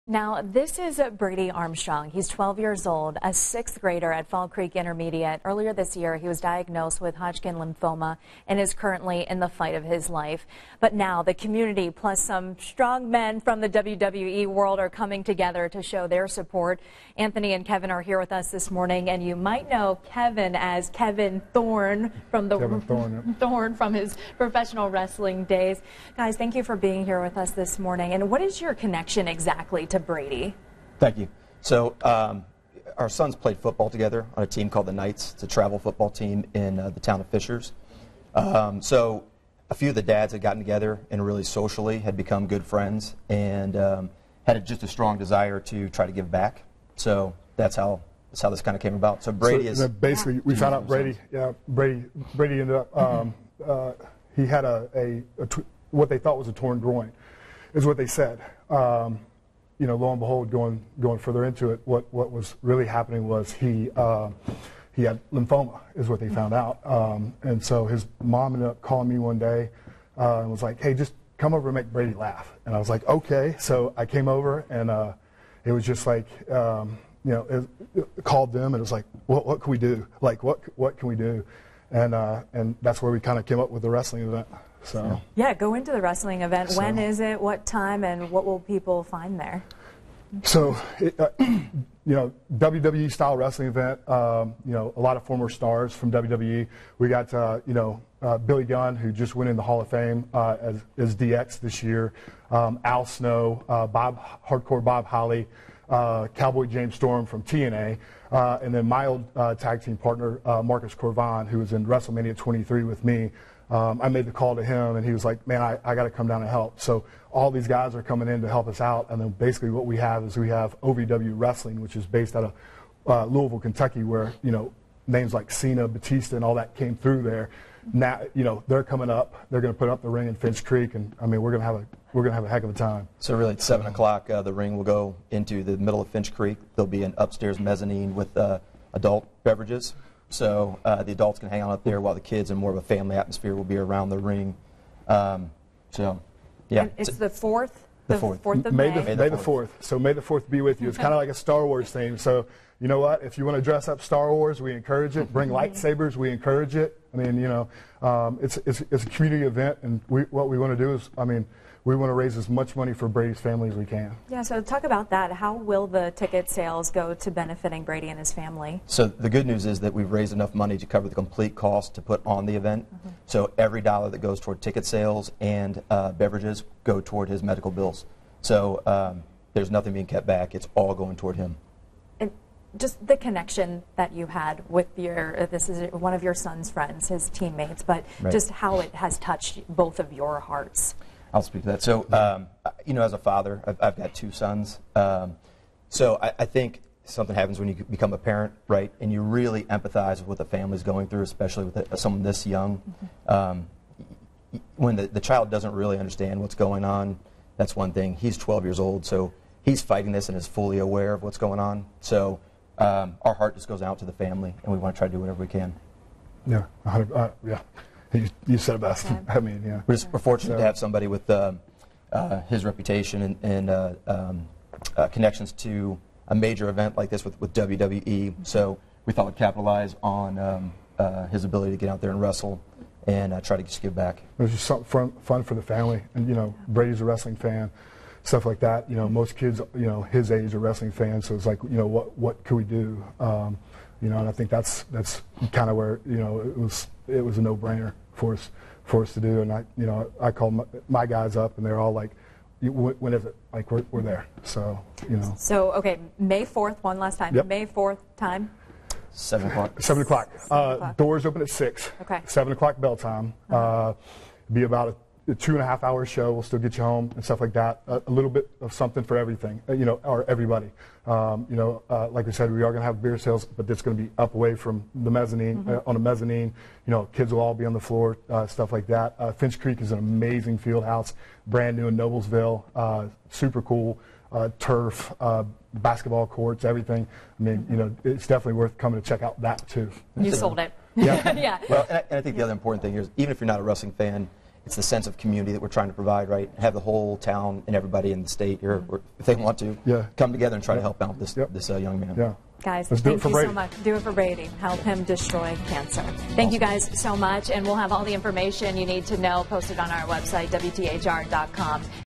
Brady Destroys Cancer
Some men from the world of wrestling are coming together to help a local 6th grader in his fight against cancer.